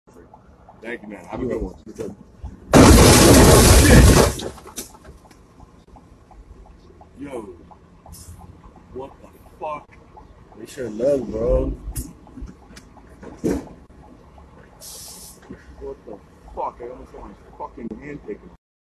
You Should Have LOOKED BRO - Funny Videos 2022
two friends have someone crash into their car, you should have looked bro...